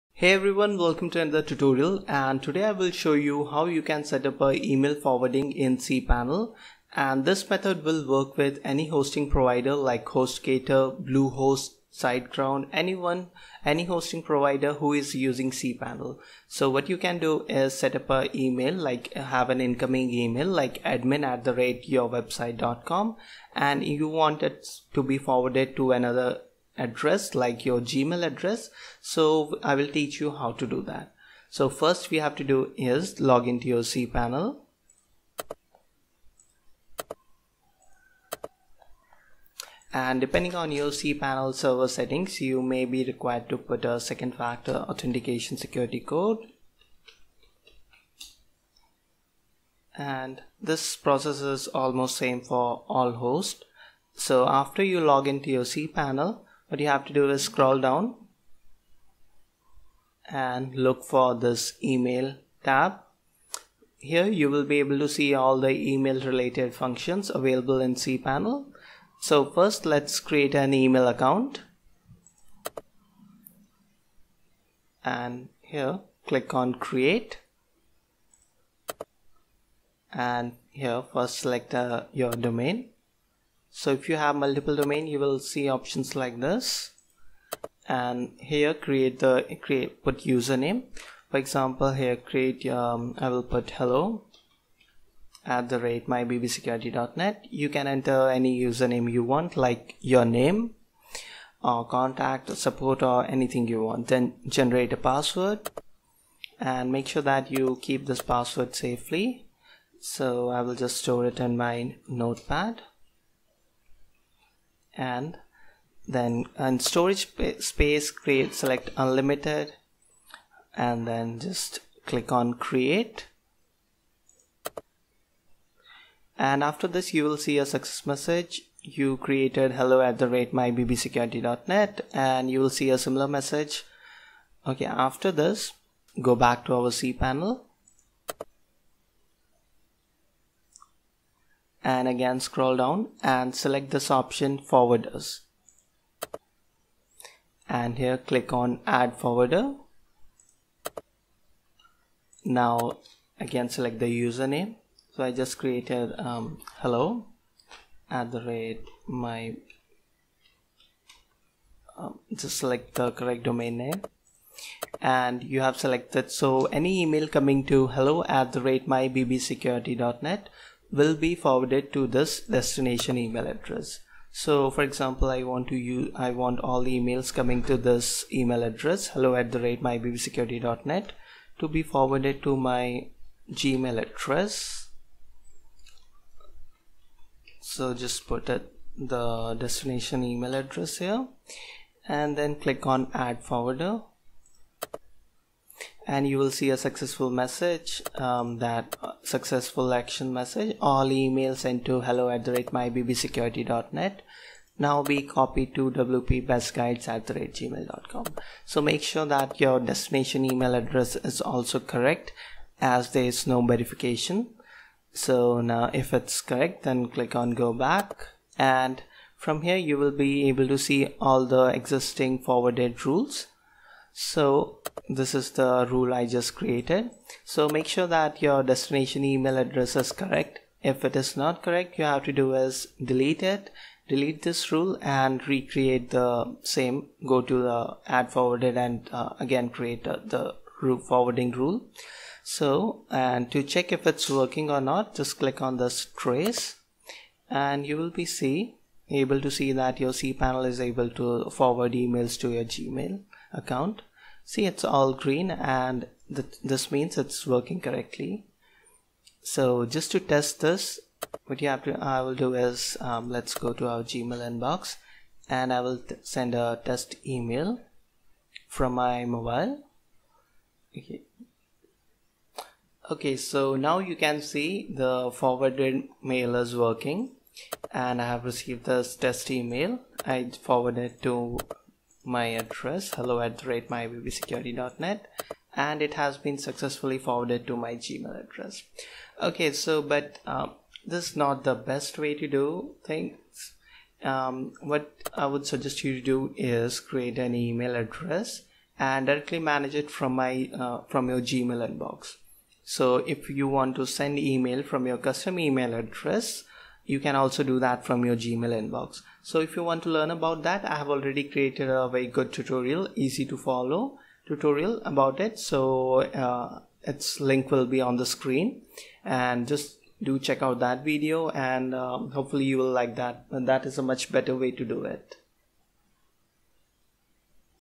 How to Setup Email forwarding in cpanel/webmail to Forward your emails to Gmail
Learn how to create an email account in cpanel using your domain and setup forwarding rule in webmail to forward it to your gmail account or any other email account.